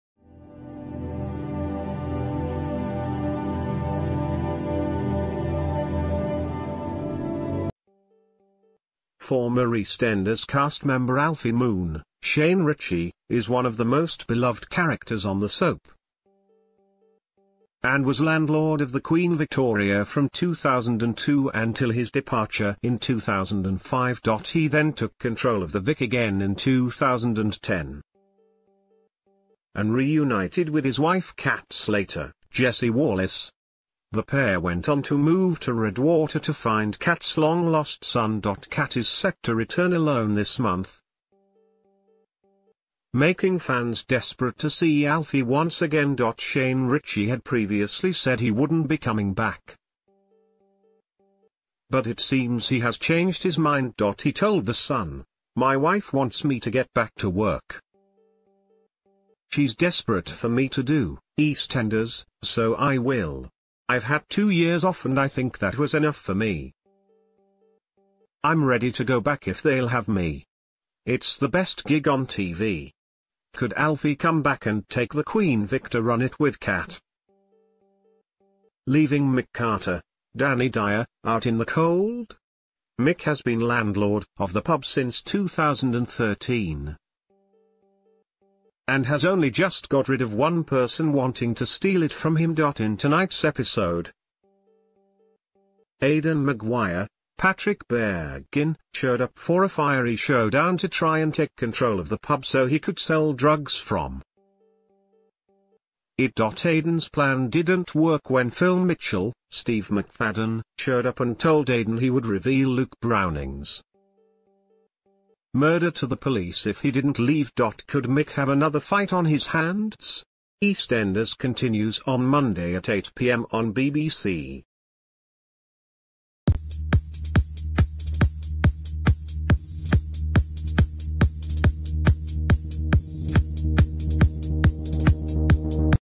EastEnders spoilers: Legendary character returns for epic Queen Vic battle?
Former EastEnders cast member Alfie Moon (Shane Richie) is one of the most beloved characters on the soap, and was landlord of the Queen Victoria from 2002 until his departure in 2005.He then took con